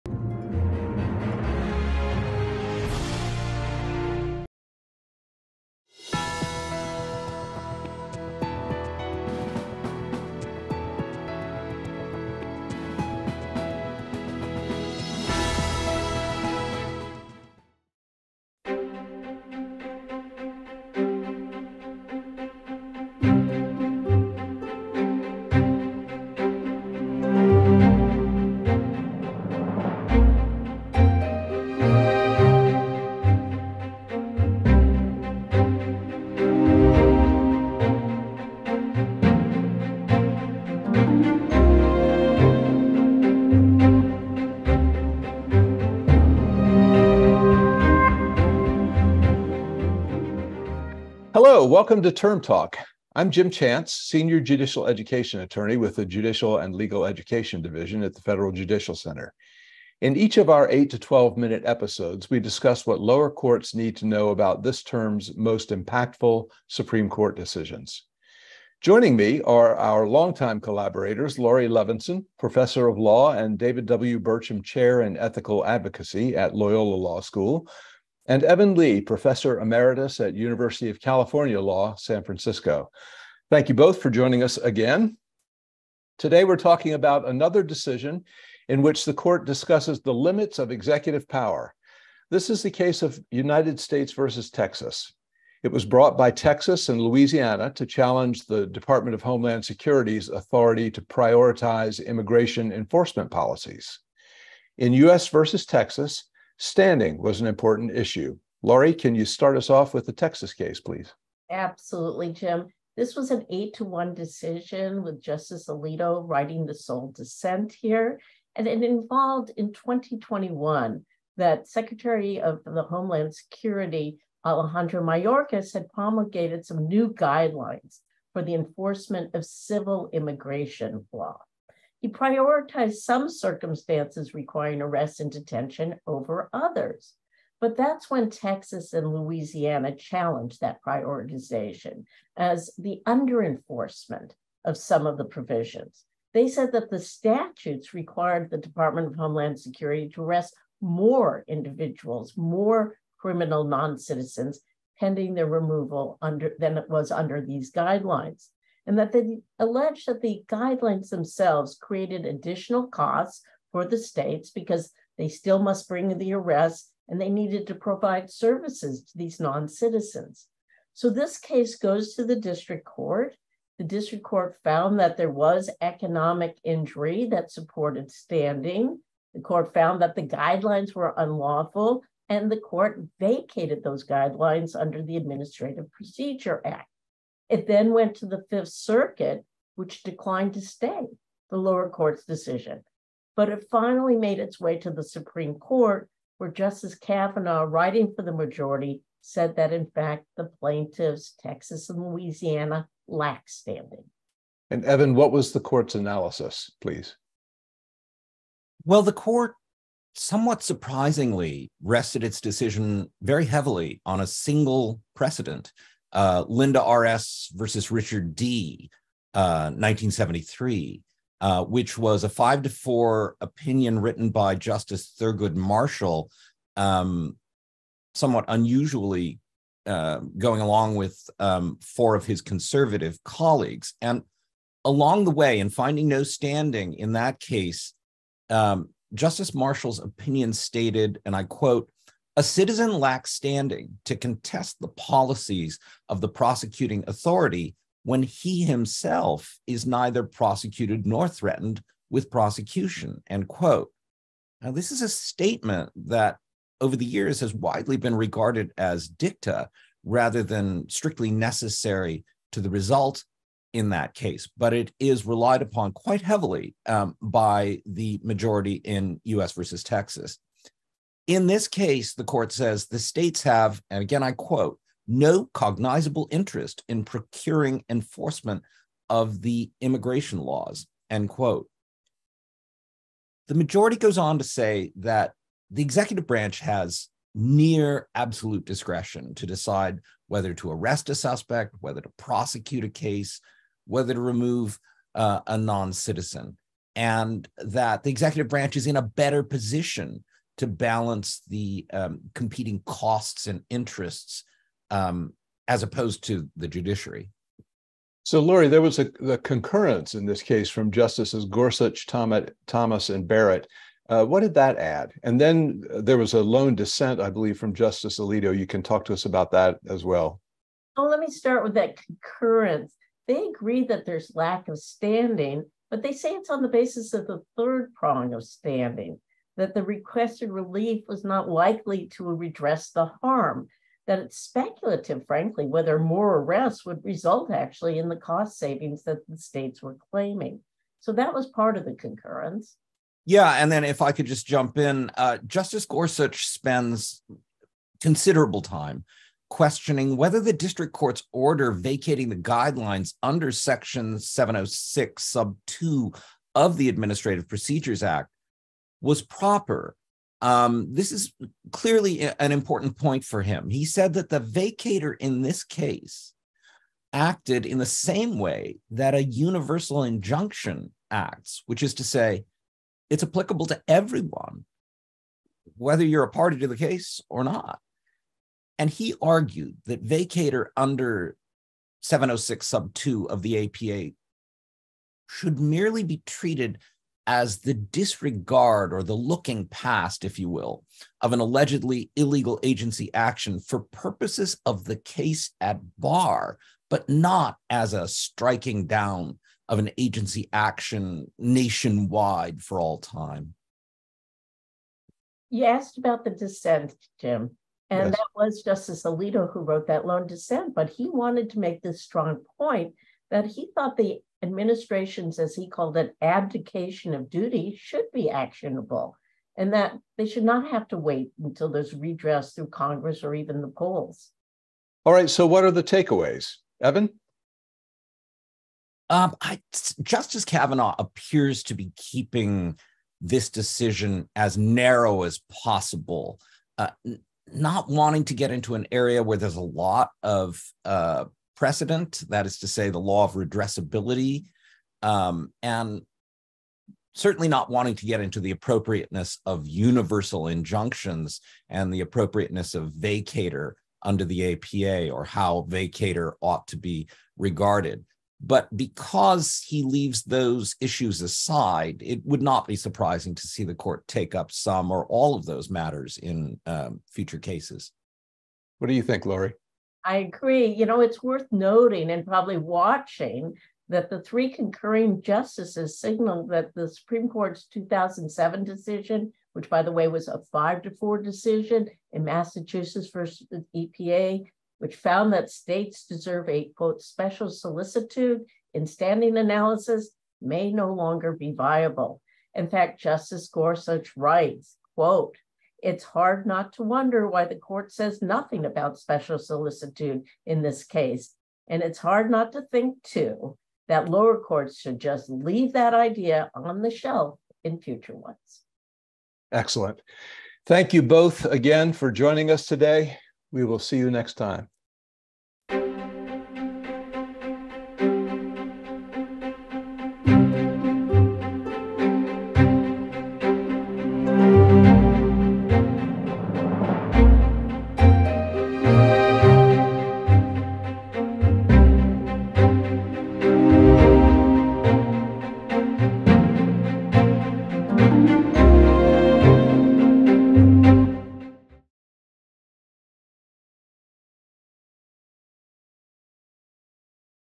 Term Talk (2022-2023): United States v. Texas
Executive Power. Major Questions Doctrine. Standing. Experts discuss the Court’s elevated clear statement rule in Biden v. Nebraska, and a potentially diminishing special solicitude toward state standing in United States v. Texas.

The nation's top legal scholars discuss what federal judges need to know about the U.S. Supreme Court's most impactful decisions.